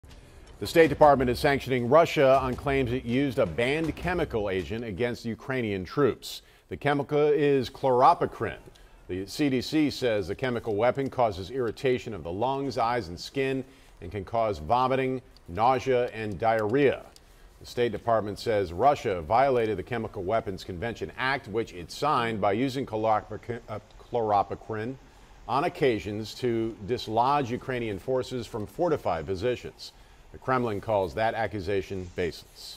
US says Russia used banned chemical agent against Ukrainian troops
The United States has formally accused Russia of using chemical weapons "as a method of warfare" against Ukraine. FULL STORY